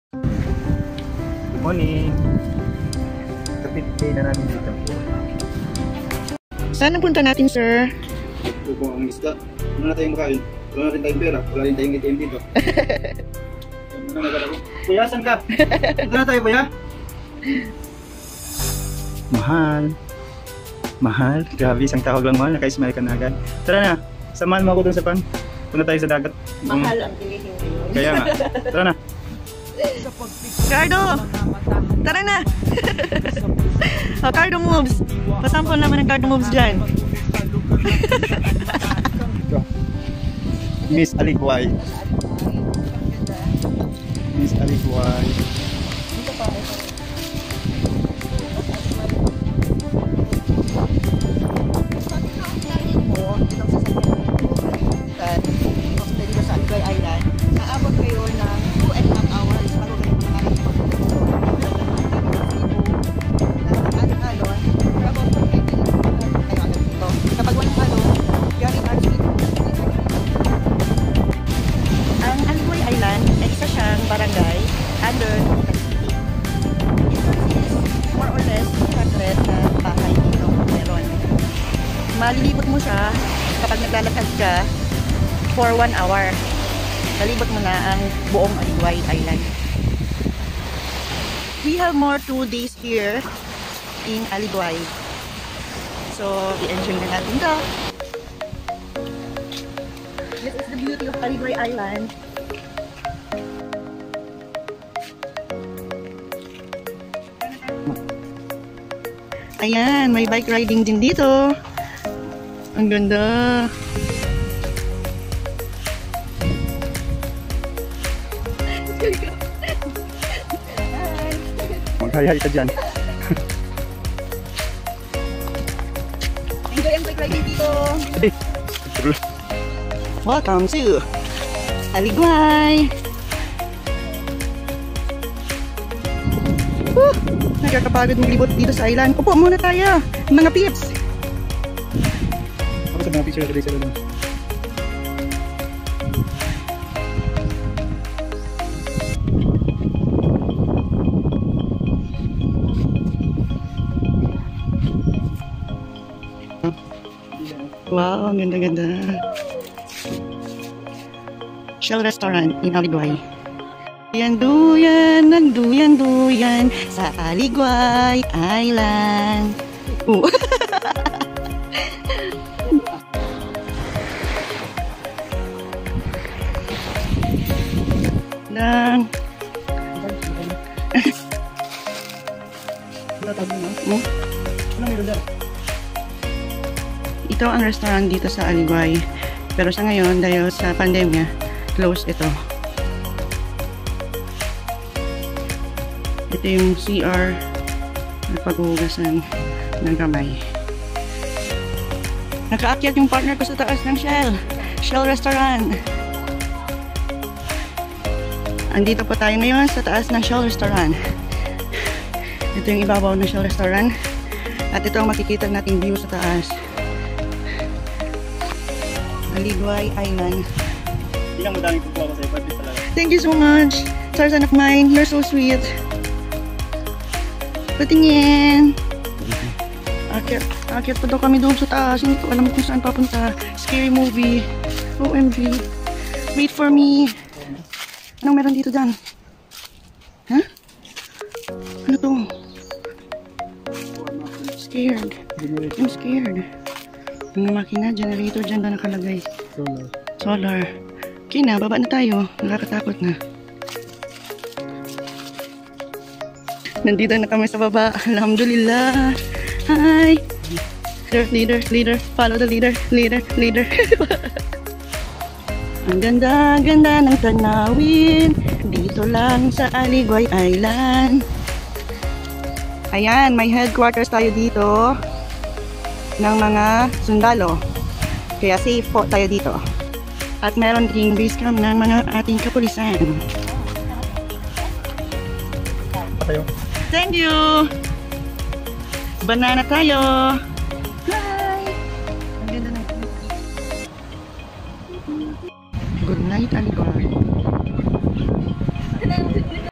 Swing/ Aliguay Island/ Day 5 in 7 Day/ Beach Resort/ Dapitan City🌊
💜🏖️🍹Day 5 in 7 Day sa Aliguay Island, White sand beach resort in Dapitan🌈⛱️

Keep safe everyone and God bless😇

👉Day 3 in 7 Day sa Isla ng Aliguay🌅

👉Day 2 in 7 Day in Aliguay Island🌅

👉Day 1 in 7 Day sa Isla ng Aliguay💕